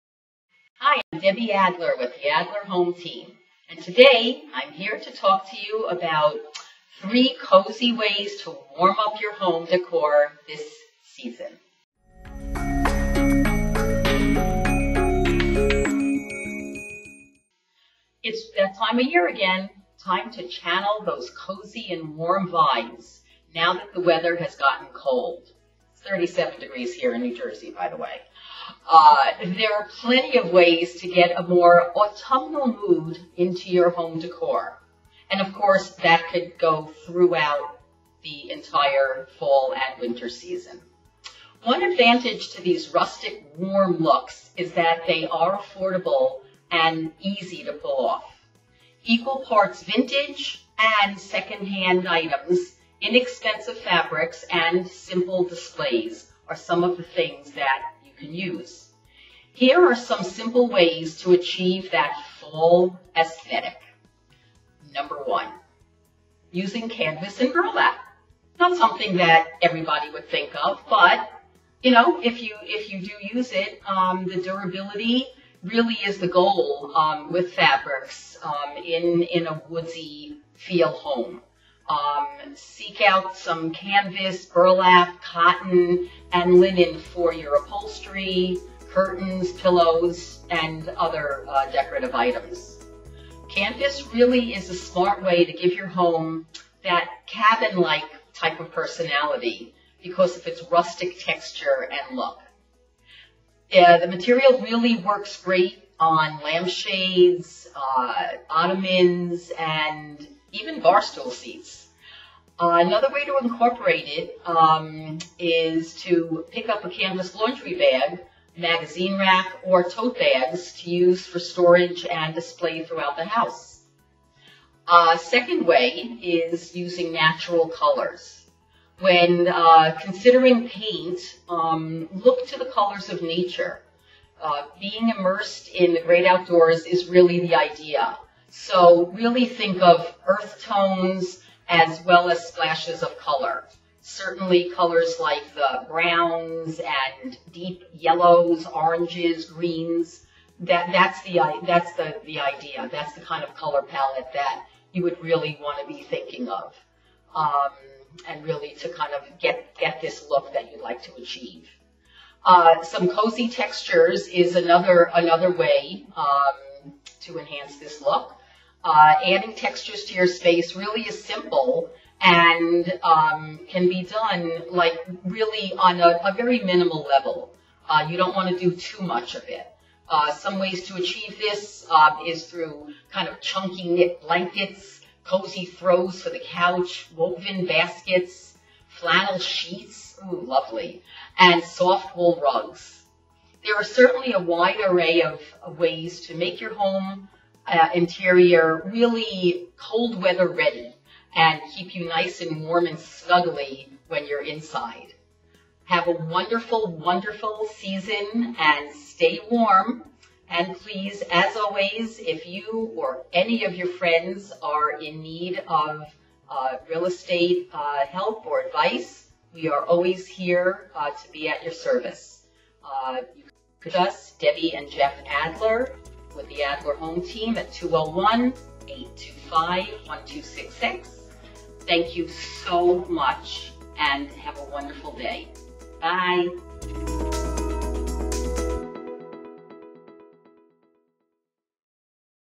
How Can You Use Home Decor to Make Things More Cozy?
As the weather gets colder, a lot of us are looking for some ways to update our home decor to give a more cozy, inviting feel. I’ve got a few different tips for you today that will help you accomplish that without breaking the bank. For example, using canvas and burlap will give your home a more rustic feel. Natural colors like brown, deep yellow, and orange is a great pallet to start with. To learn more, watch this short video.

Buying in Northern New Jersey?

Selling in Northern New Jersey?

Jeffrey & Debra Adler
Broker & Sales Associates

Keller Williams Village Square Realty